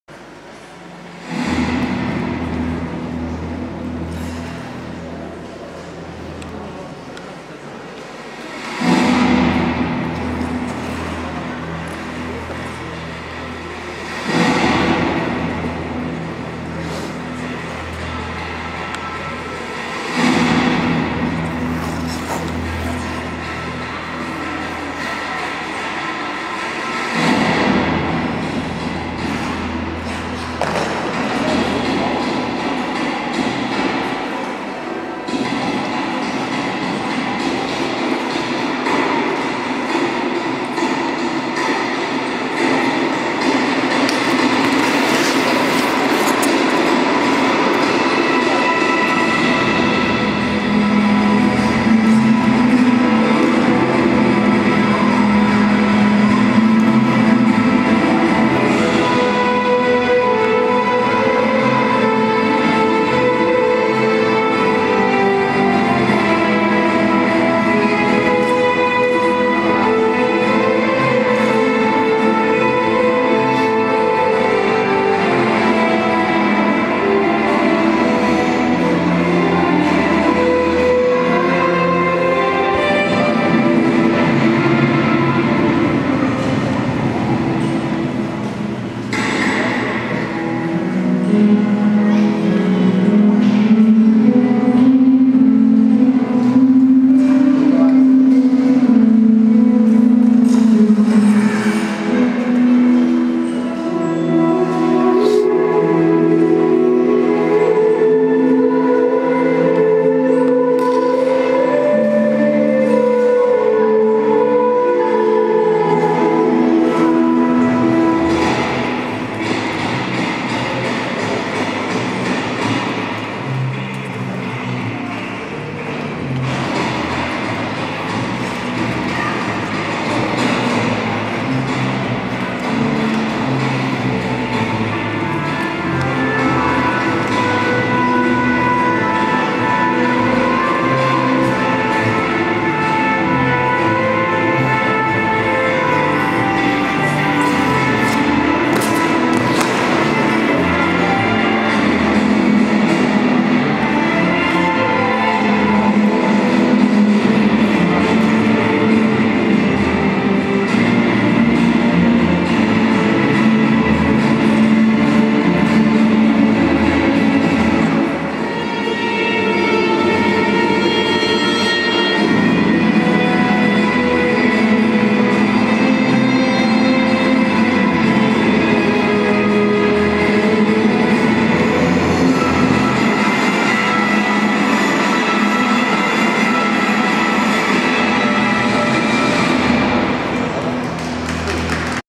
Ekaterina KRASILOVSKAYA ПП
7 - 9 ноября 2012 г., в Донецке состоялся второй этап Кубка Украины по фигурному катанию на коньках среди юношей.